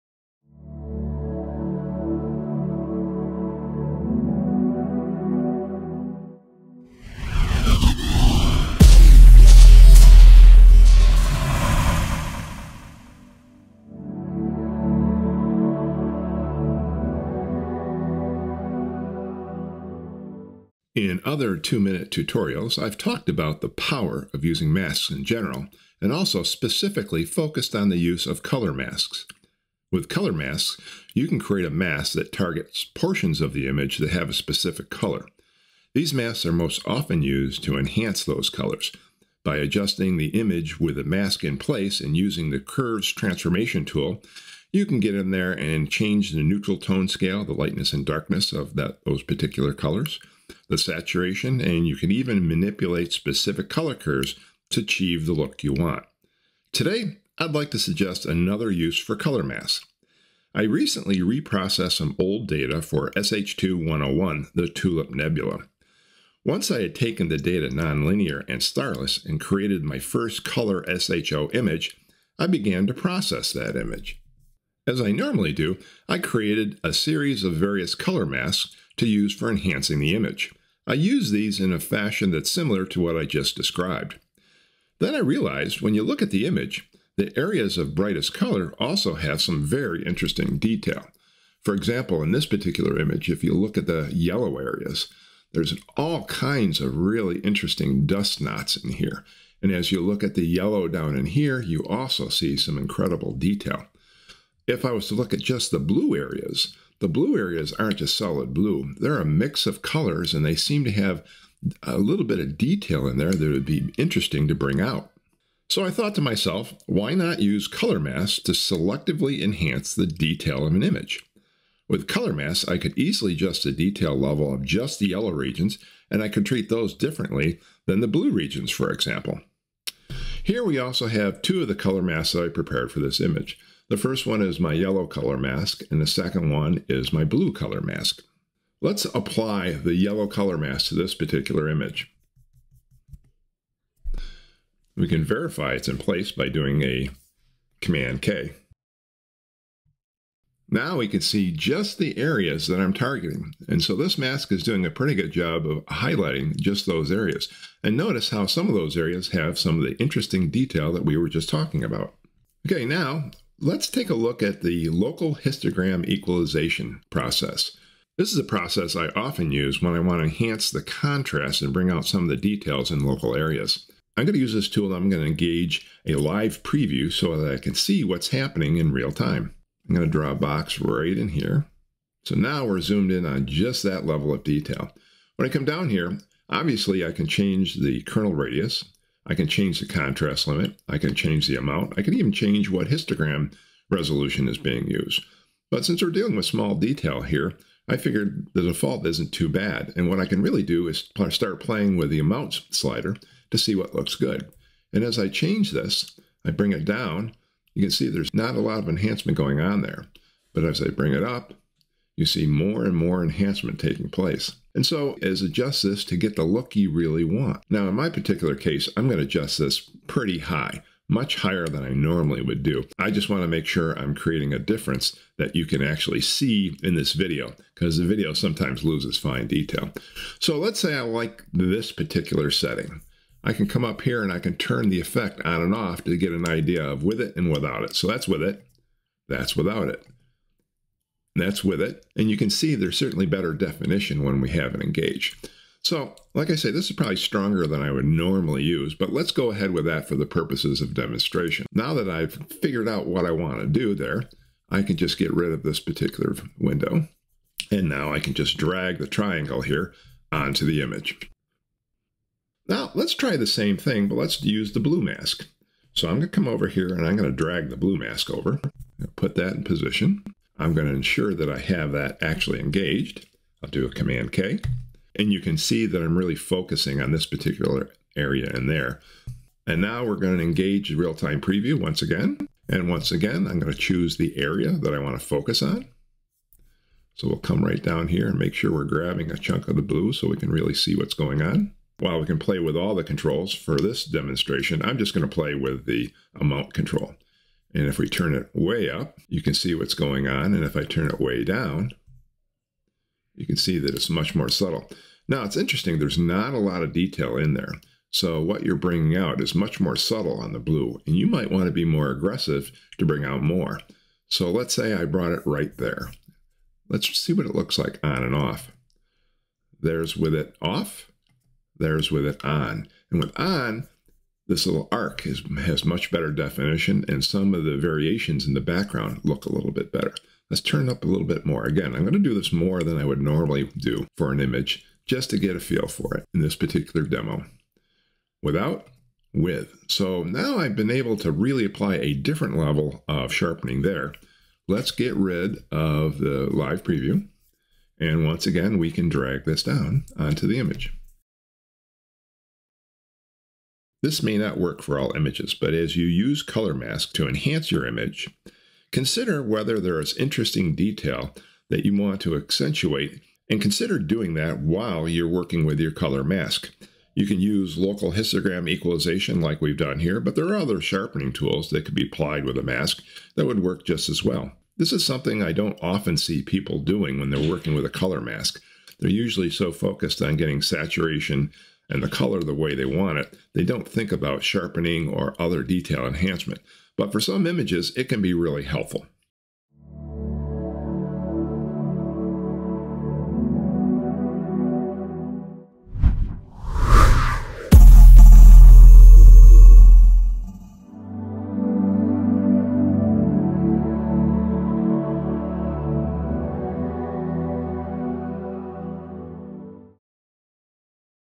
2-Minute Tutorials: Using Color Masks to Enhance Image Details!
This is another in a series of concise tutorials on working with Pixinsight.   It is geared towards new astrophotographers and those just learning Pixinsight.

This video covers the use of Color Masks to selectively enhance the details of an image.

Color Masks are commonly used to target enhancements of color in an image - often using the CurvesTransformation tool.  But in many images, areas of high color also have interesting detail associated with them.  This video shows how you can use color masks to selectively and differently enhance details in these color areas.

There you will find images, gear, tips, and techniques!

Is there a 2-minute tutorial that you would like to see?

Thanks,
Pat